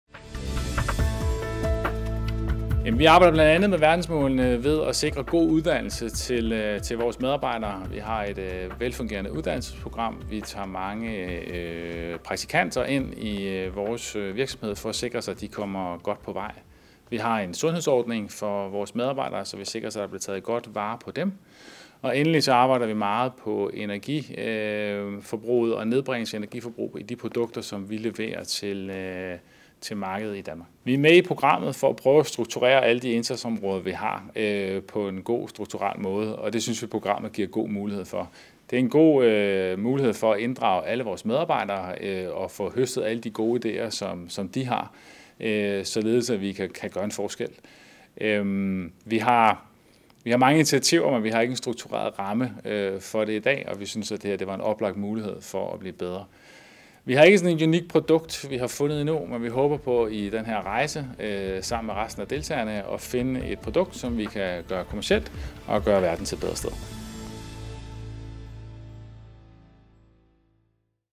Bæredygtig forretning i regi af FN's Verdensmål CASA AS - bygger på fremtiden
CASA vil forsøge at skabe ny bæredygtig forretning i regi af FN’s verdensmål.

I 2015 blev alle FN’s medlemslande enige om 17 mål, der opregner klodens største miljømæssige, økonomiske og sociale udfordringer. I indsatsen for at nå de 17 Verdensmål spiller den private sektor en helt afgørende rolle. Dette anerkender CASA, der deltager i et nyt FN-projekt, hvor målet er at skabe ny bæredygtig forretning i regi af FN’s Verdensmål. CASA A/S med kontor i Horsens og København er en entreprenør- og udviklingsvirksomhed med byggeprojekter over hele Danmark.